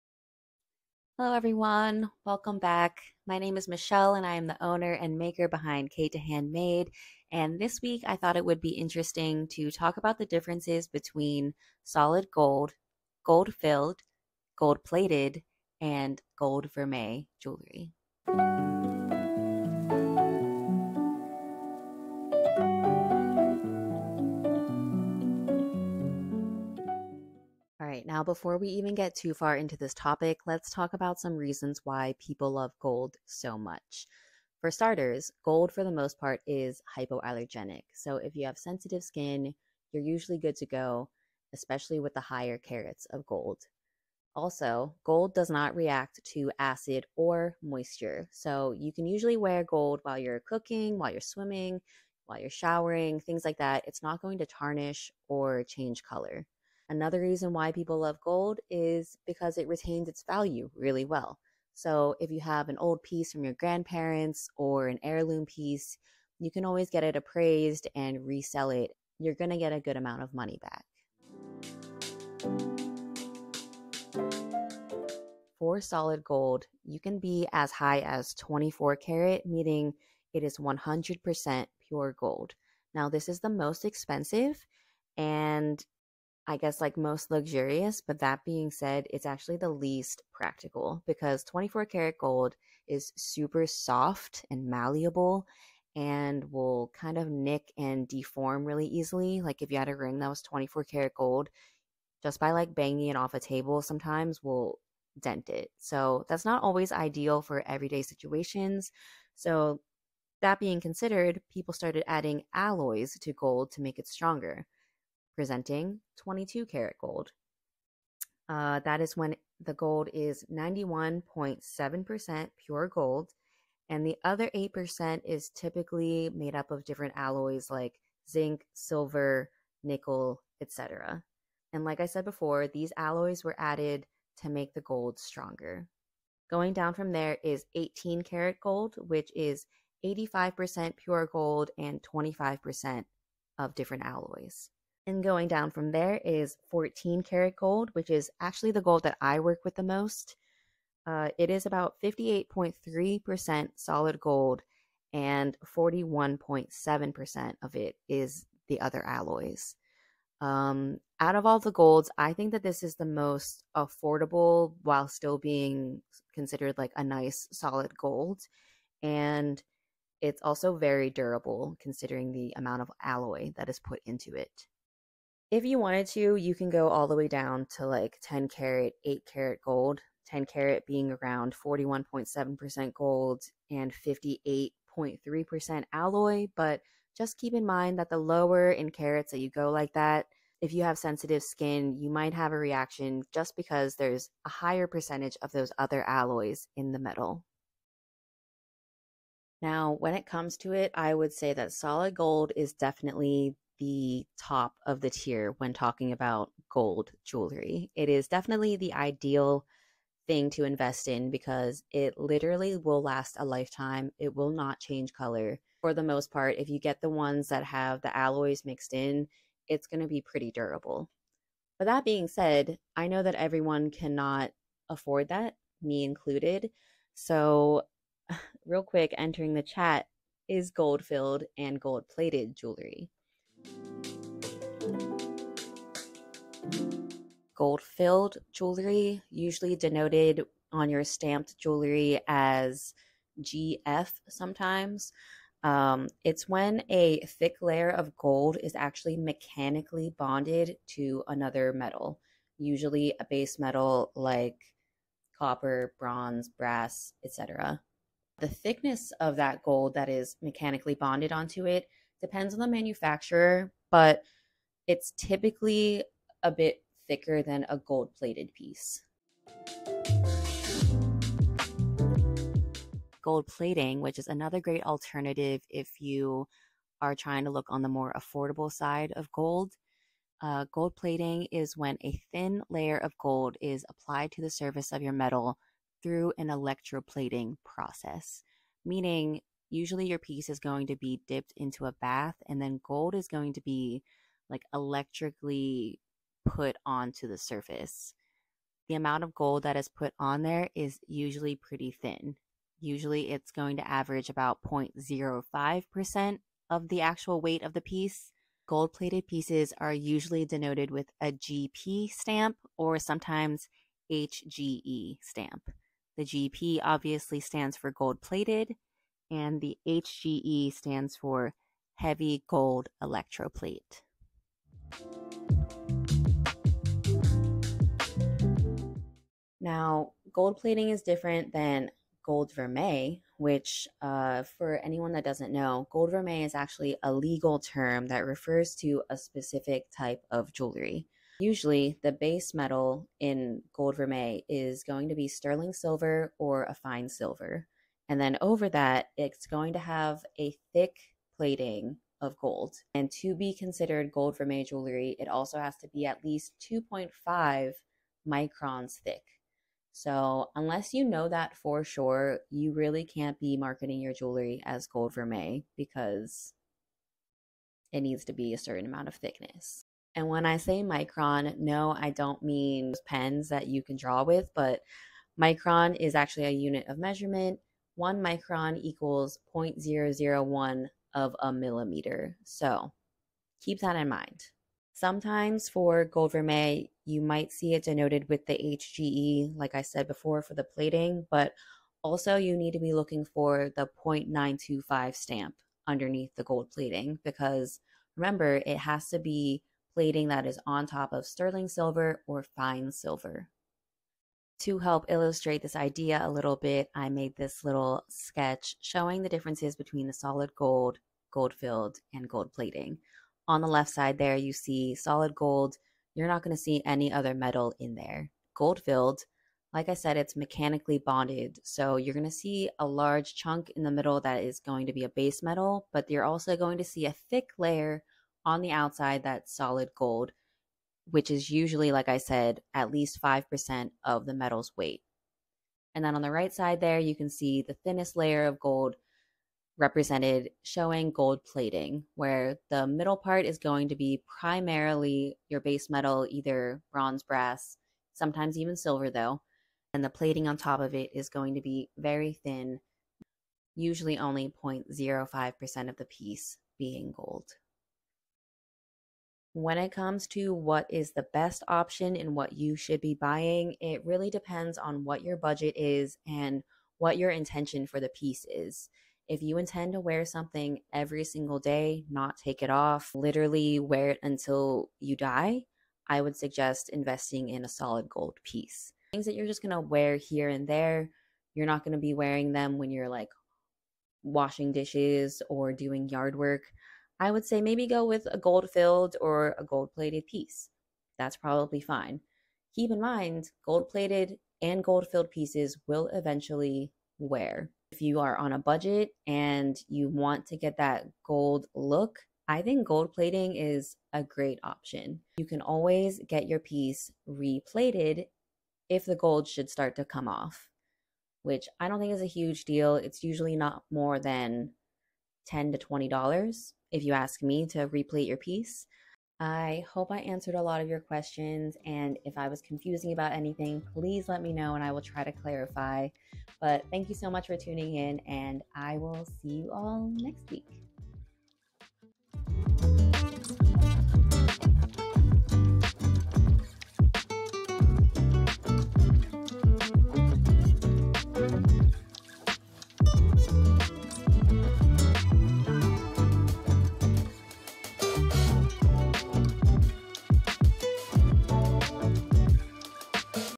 Explaining different types of GOLD jewelry
Hi!  My name is Michelle and I'm a full time jeweler and artist.
Let me explain the differences between solid gold, gold filled, gold plated, and gold vermeil jewelry.

(Sorry that I keep looking down to reference my notes... I am trying to be better about talking directly to the camera, but I didn't want to say any of these details wrong.)

Thanks for being here!  Let me know if you have any questions and follow along for more :)